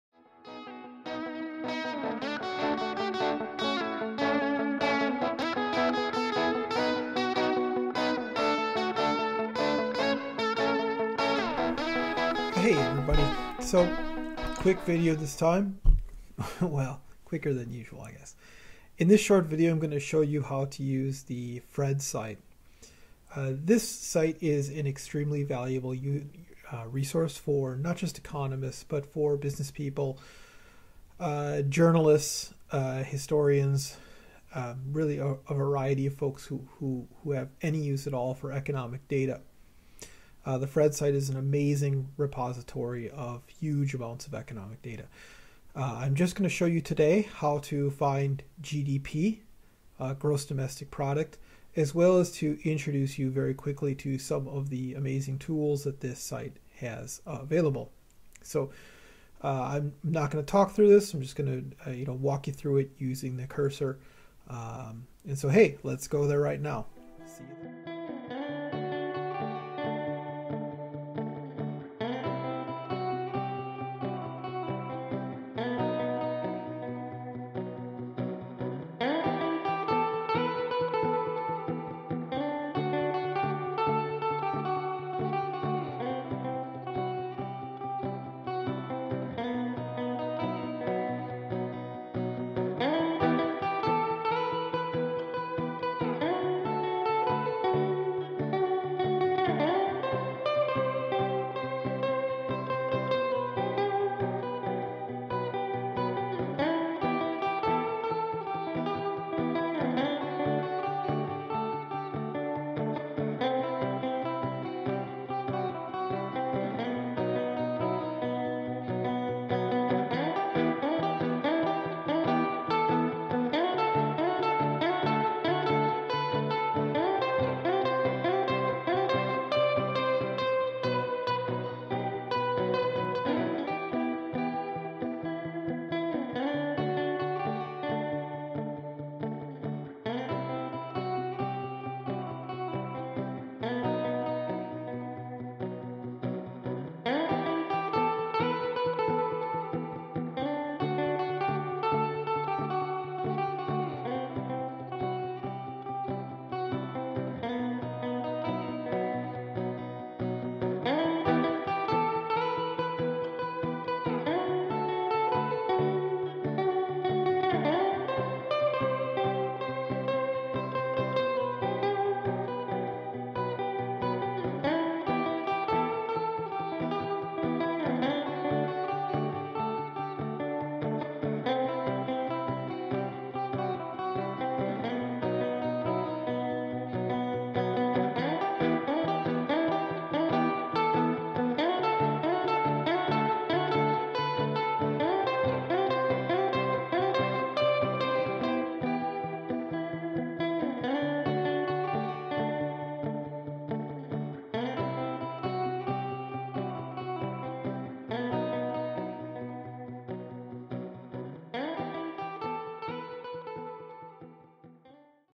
How to Find Reliable Economic Data -- Fast and Free: GDP
An introduction to using the Economic Database at the St. Louis Federal Reserve. (FRED) Speaker/Guitars: Dr. Thomas Kemp  U of Wisconsin - Eau Claire